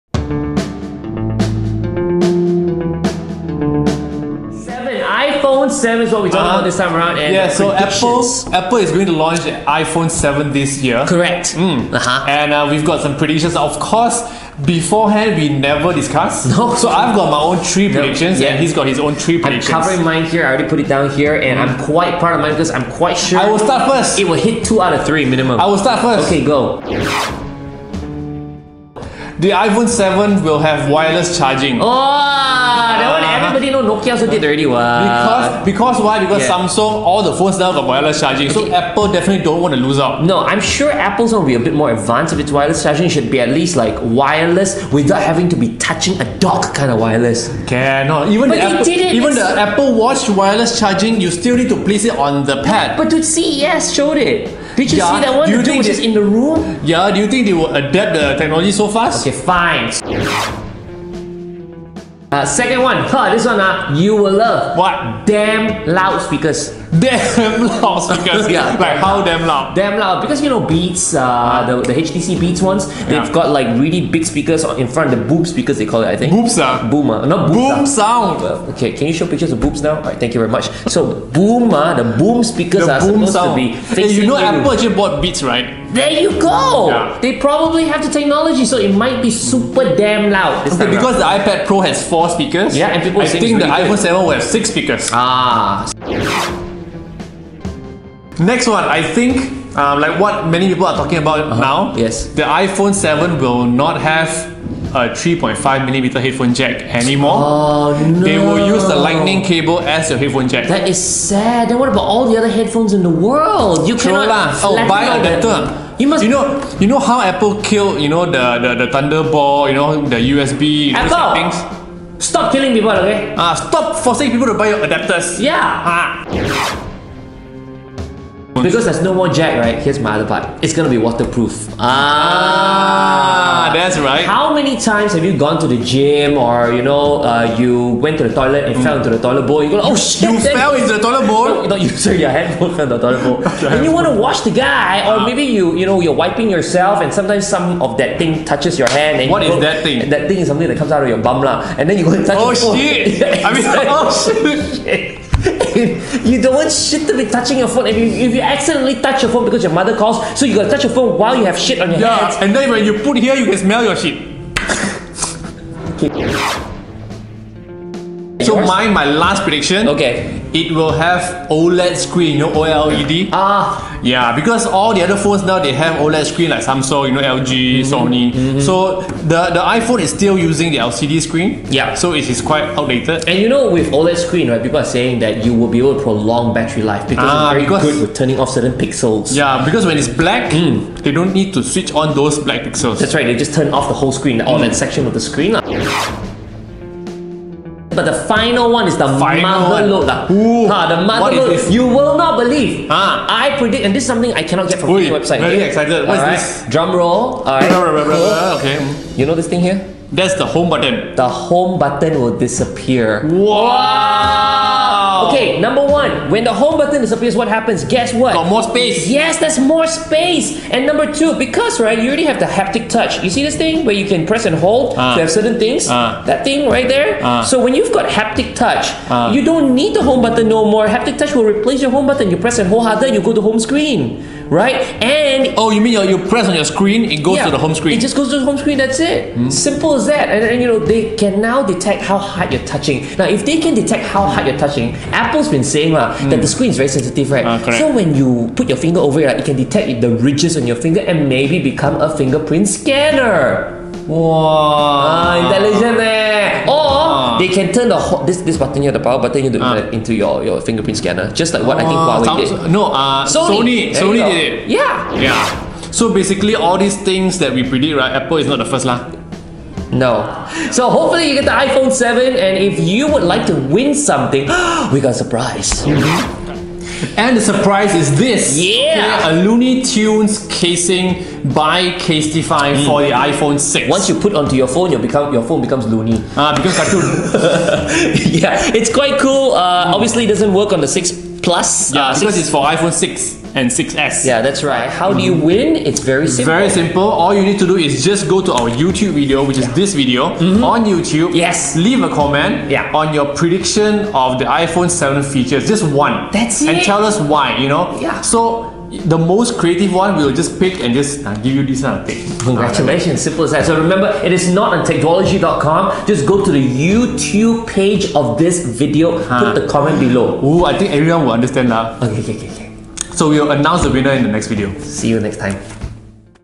Techduology S3E4 - iPhone 7 Features Predictions
Want to know what features will come with the iPhone 7?
Check out predictions from @smashpop & @benjern!

smashpOp & Ben check out the DJI Osmo, a new handheld camera that shoots 12MP photos and 4K video while being stabilized with a 3-axis brushless gimbal.